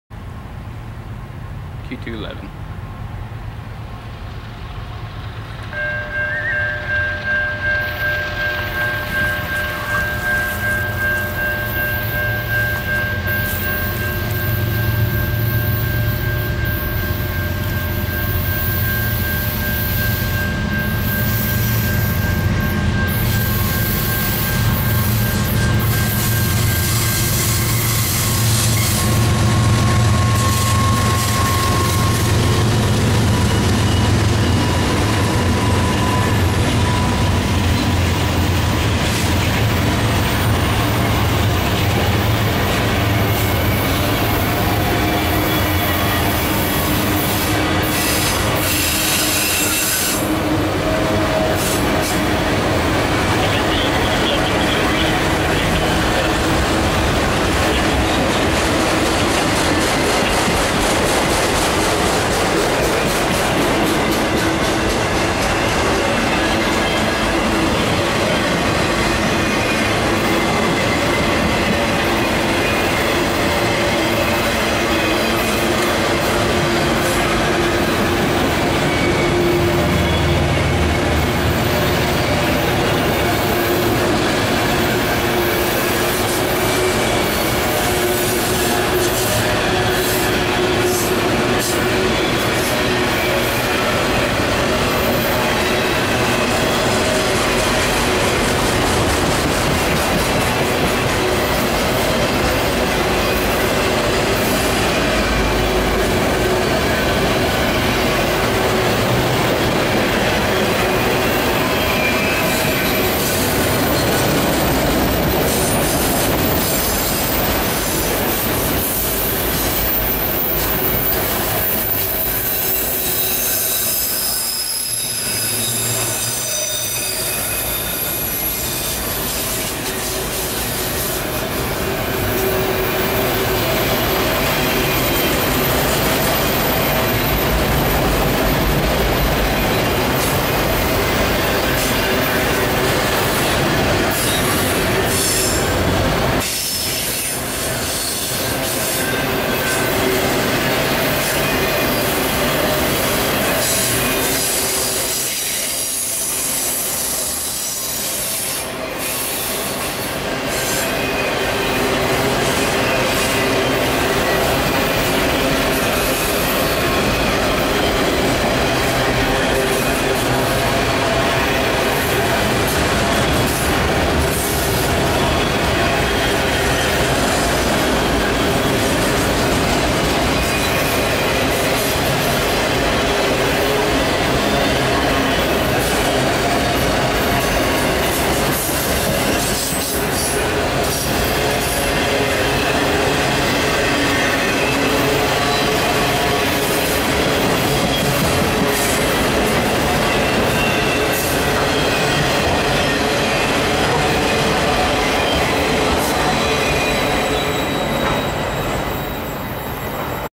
CSX 5327 leads CSX Q211-22 in Smyrna, GA
Milepost OWA 15.0
4/23/13
6:44p.m.
NB CSX Autorack Train Q211 steps thru Smyrna, GA as it glides down hill to Atlanta. Please Enjoy!
CSX: Q211-22
CSX: 5327(ES40DC)
CSX: 9032(CW40-9)
Southbound
Autorack Train